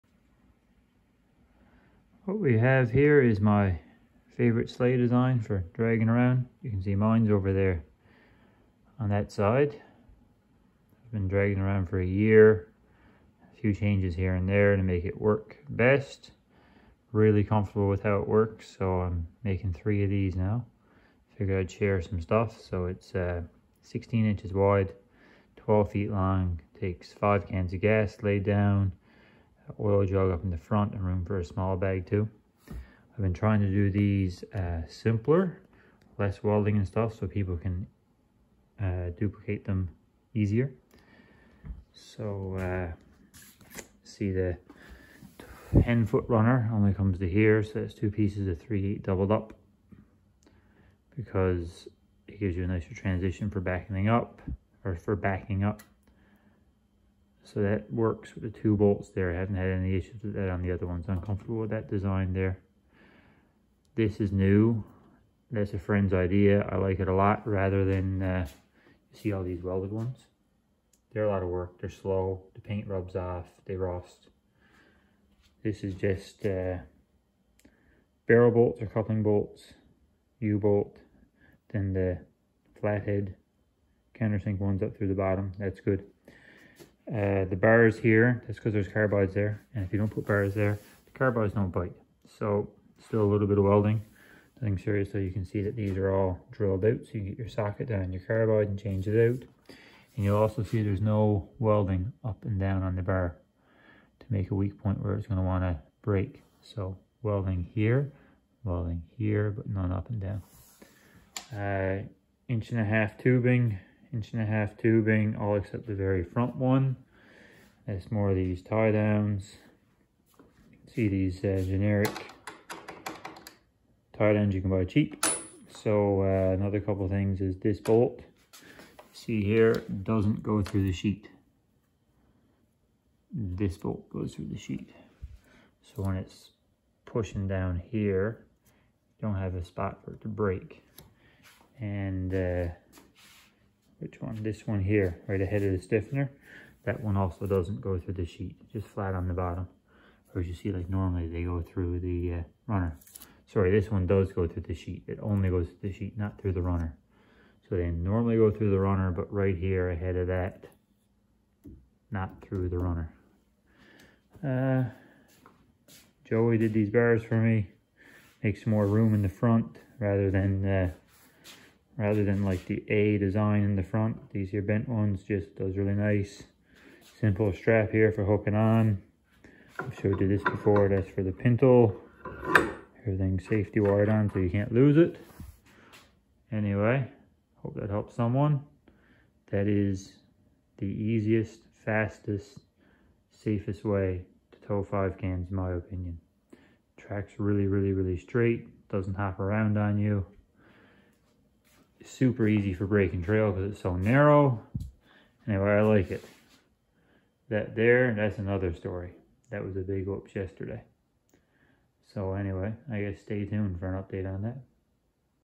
My Favorite Sleigh
**Part way through the video I say a bolt doesn't go through the sheet on the front bar. It DOES go through the sheet. I meant to say it doesn't go through the RUNNER. Opps.

Specks are:

12'x16" sheet
2 outside runners are 3"x3/4"
Middle runner is 2"x3/4"
Stiffener strips on top are 2"x3/4" beveled at 45 degrees to 1 1/2" on top.
Bolts are all grade 8, 5/16" except a couple in tiedowns only intended for bags.
Front bars are 22" long, 9 1/2" from the front bars to the first cross bar. That one is 1" square. All the other cross bars are 1 1/2" square. All cross bars have the radiused corners.